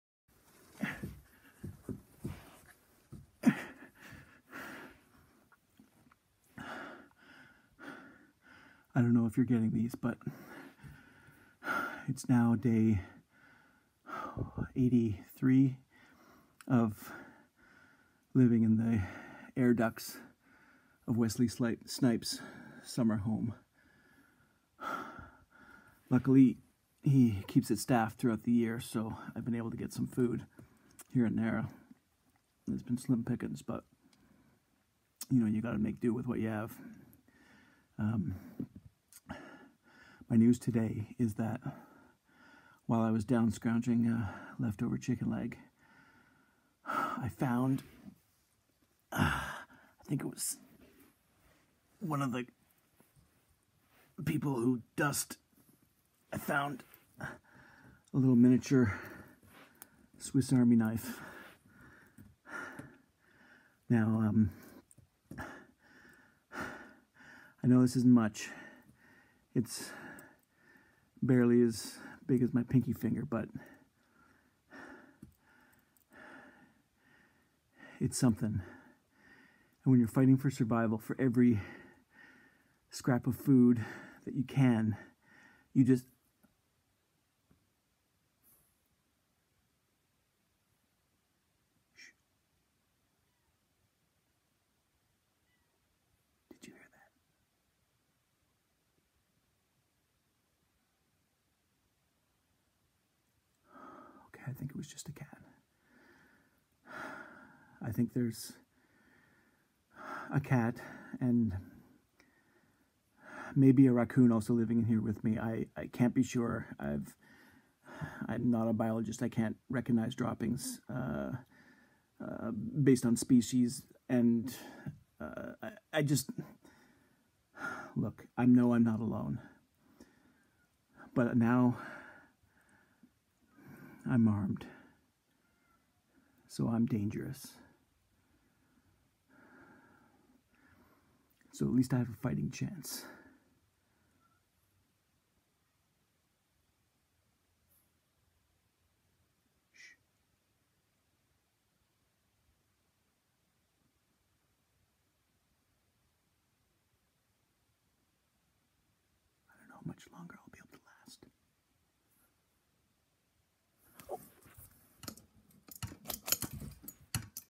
Daily Character #31 (2020-05-01)
I'm an actor/comedian/improviser and what I've been doing to feed my creative impulses during the pandemic lockdown is to improvise a short character monologue every day. Enjoy!